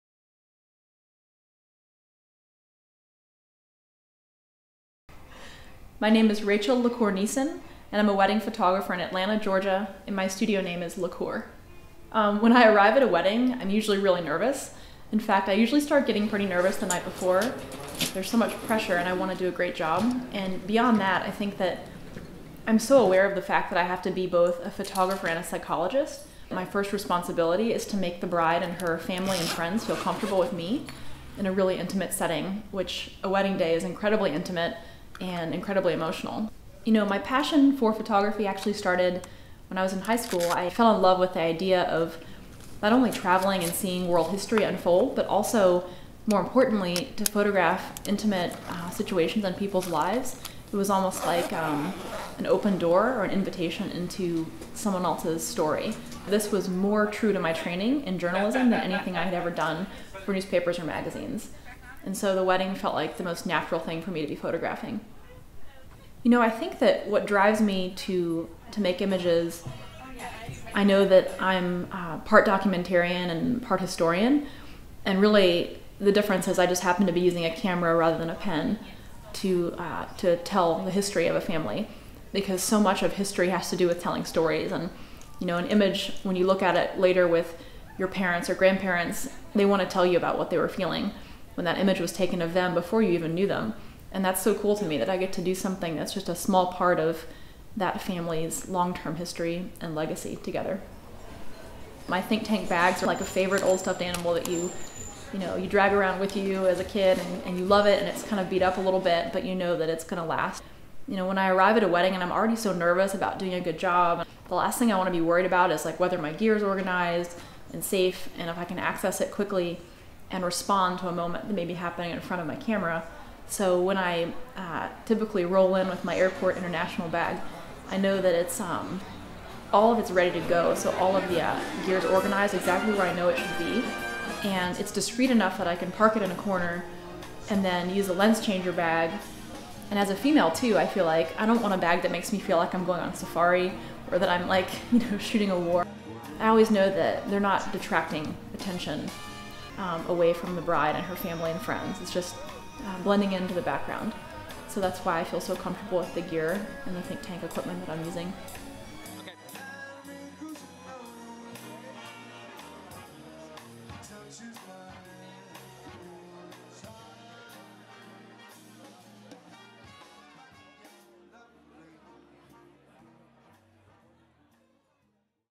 Rachel LaCour Niesen--Think Tank Photo Design Board Member
Think Tank Photo Design Board member Rachel LaCour Niesen talks about her work as a wedding photographer and how Think Tank Photo camera bags are and integral part of her workflow.  With a background in photojournalism, Rachel provides storytelling imagery for her clients to preserve some of the most emotional and memorable moments of that family's history.  LaCour, the wedding photography company Rachel started with Andrew Niesen and Mark Adams, was named among the "Top Ten Wedding Photographers in the World" by American Photo magazine.  (video by Andrew Niesen; edited by Kurt Rogers)

To see more work from LaCour, please visit their

For more information on the Think Tank Photo products Rachel uses in the video, other Think Tank Products and the Design Board, please click on one of the following links: